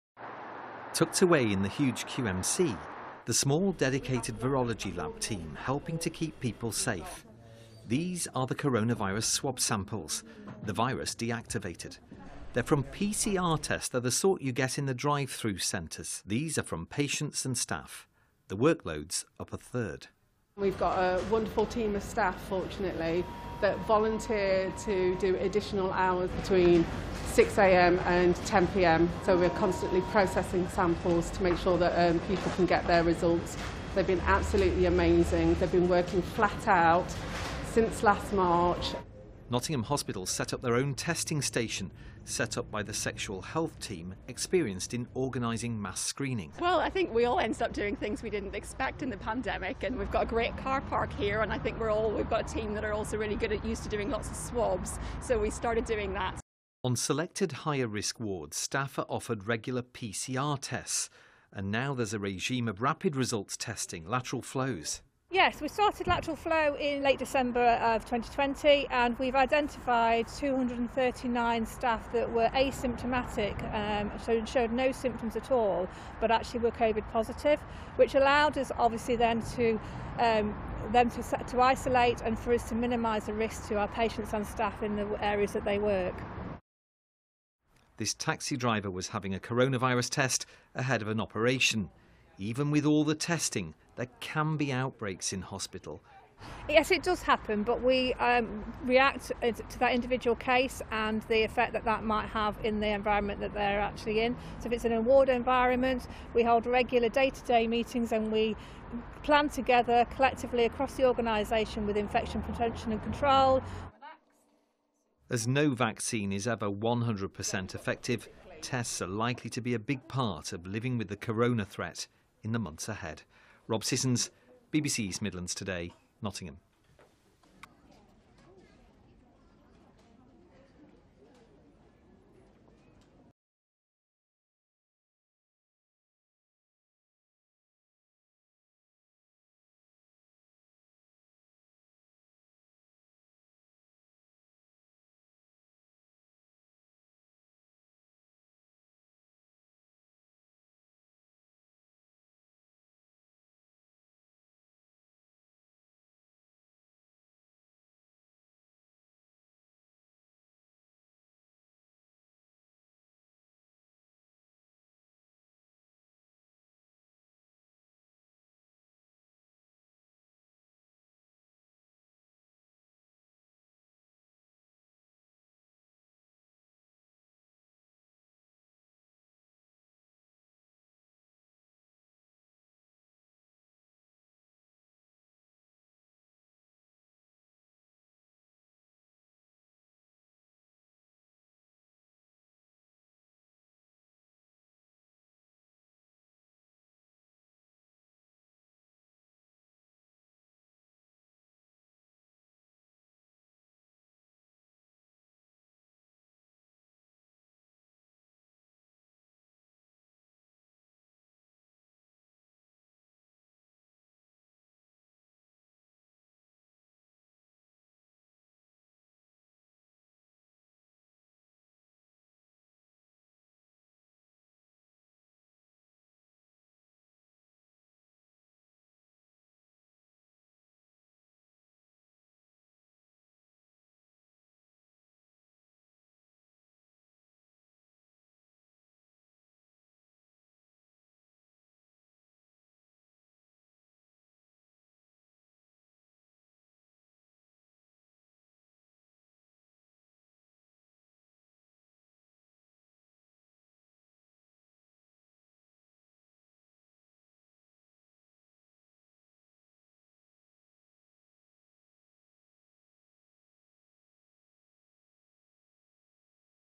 Video: Behind the scenes at Nottingham's QMC Covid testing laboratory
The Microbiology team at Nottingham University Hospitals, which sits in West Block at the Queen’s Medical Centre, have been the hidden heroes driving forward the Trust’s testing machine during the Covid-19 pandemic.

The laboratory, led by Dr Annie Joseph, Laboratory Head of Service and Rebecca Walker, Service Manager, was ahead of the curve from the very start of the first wave of the pandemic, being set up for business at the end of March.